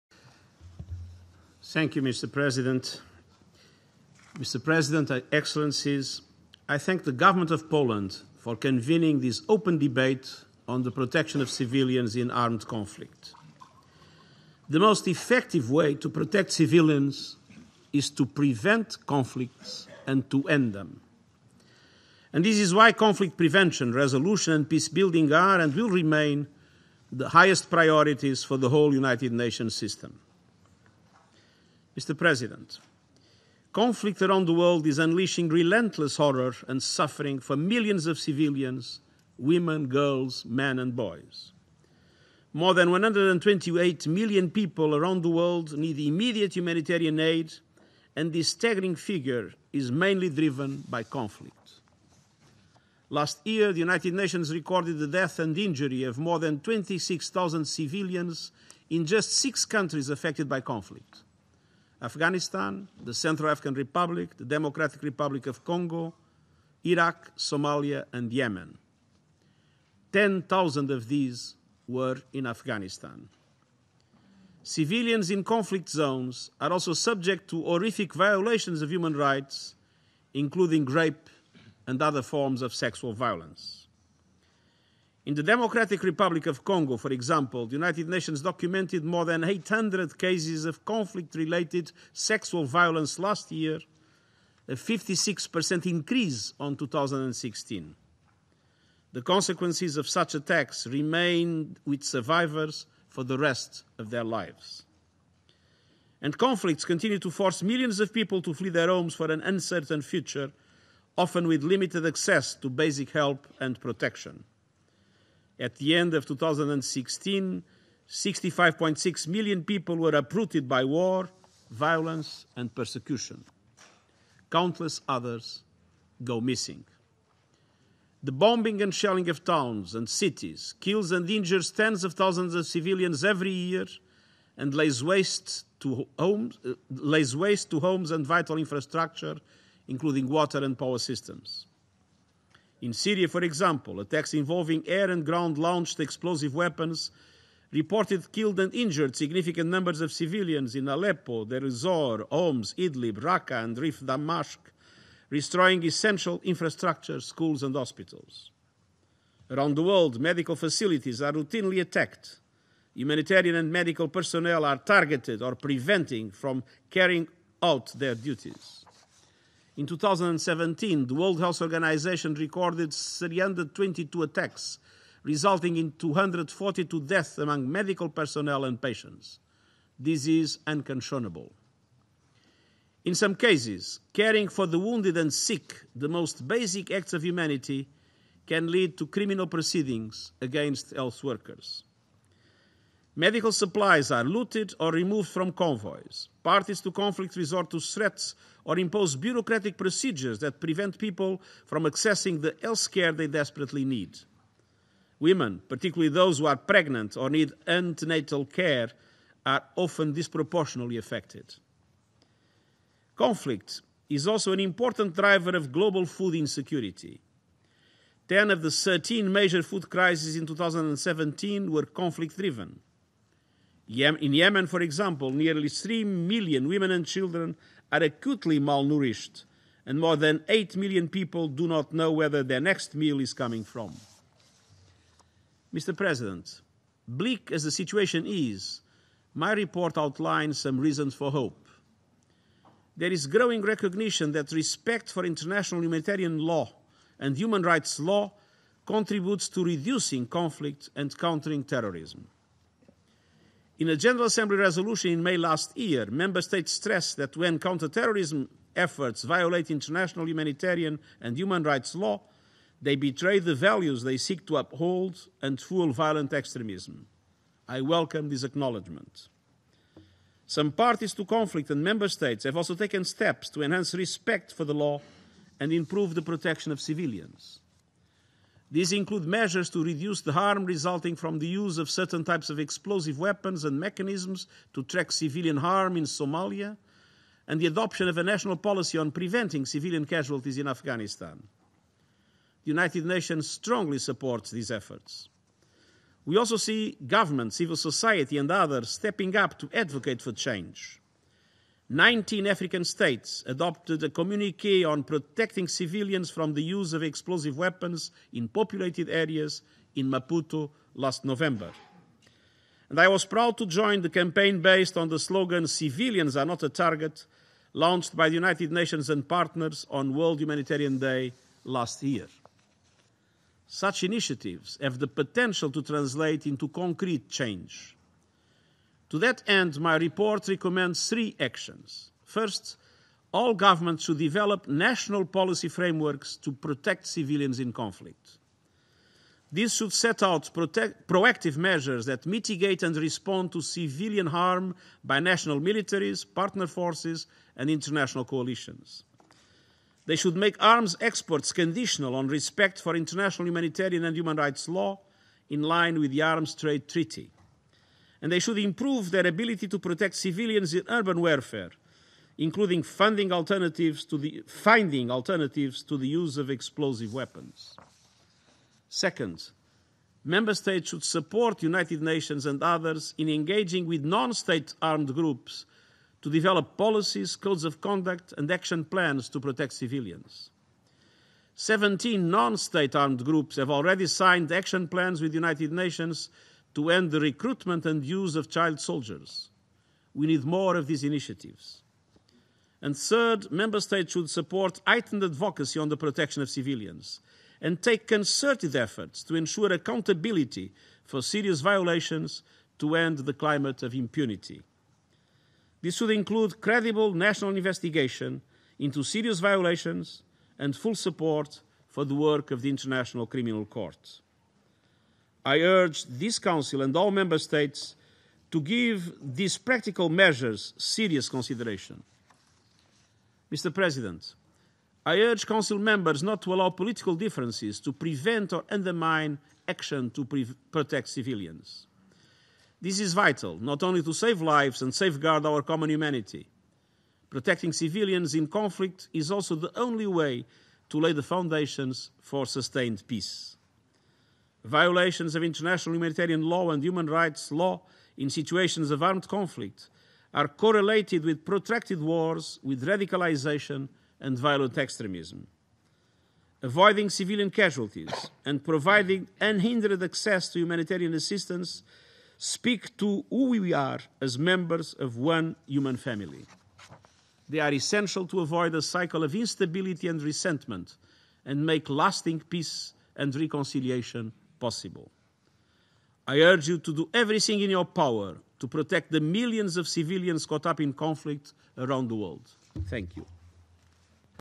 The protection of civilians in armed conflict - António Guterres at the UN Security Council
Remarks by H.E. Mr. António Guterres, United Nations Secretary-General, at the Security Council meeting on the protection of civilians in armed conflict.

UN Security Council, 8264th meeting

Addressing a Security Council debate on the protection of civilians in armed conflict, Secretary-General António Guterres today (22 May) stressed that “the most effective way to protect civilians is to prevent conflicts and to end them.”

Guterres reiterated that “conflict prevention, resolution and peacebuilding are, and will remain, the highest priorities for the whole United Nations system.”

The Secretary-General noted that “conflict around the world is unleashing relentless horror and suffering for millions of civilians, women, girls, men and boys.” He said, “more than 128 million people around the world need immediate humanitarian aid, and this staggering figure is mainly driven by conflict.”

He urged Council Members “not to allow political differences to prevent or undermine action to protect civilians.”

Guterres said this was “vital not only to save lives and safeguard our common humanity” as “protecting civilians in conflict is also the only way to lay the foundations for sustained peace.”

He called on Member States to develop national policies to protect civilians in conflict and to ensure accountability for serious violations against them.

The Director-General of the International Committee of the Red Cross (ICRC), Yves Daccord, told the Council “there is clearly still a long way to go before the various normative and policy achievements regarding protection of civilians are felt where they really matter. Not on paper, but on the ground.”

He highlighted four particular areas, namely: “To avoid the use of heavy explosive weapons in populated areas. To counter a wide range of threats to health care. To ensure humane treatment and conditions of detention for all detainees. And to commit to preventing people from going missing and properly accounting for those who do.”

The Minister of Foreign Affairs of Poland, Jacek Krzysztof, told the Council it was “important that all states and non-state parties to the conflicts comply with their legal obligations” and said it was “crucial to implement good practices in this field.”

Poland holds the presidency of the Council for the month of May.